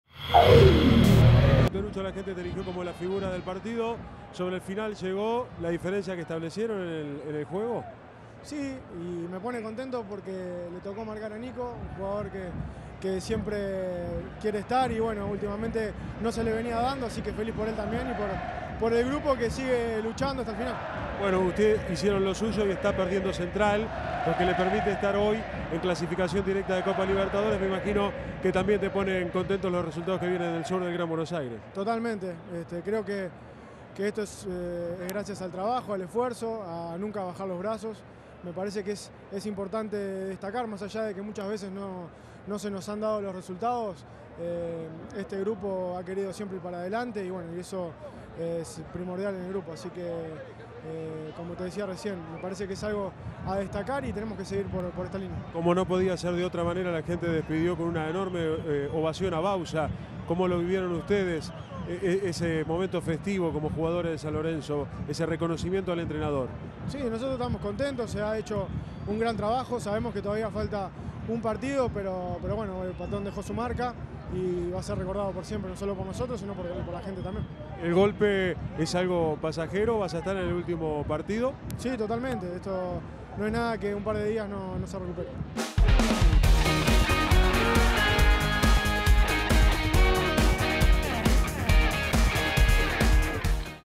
Cauteruccio:"Se hizo un gran trabajo". San Lorenzo 2 Temperley 0. Fecha 29..FPT
Martín Cauteruccio, elegido como la figura de la App Oficial FPT y autor del primer gol, destacó el esfuerzo del grupo y el trabajo de Bauza con este plantel.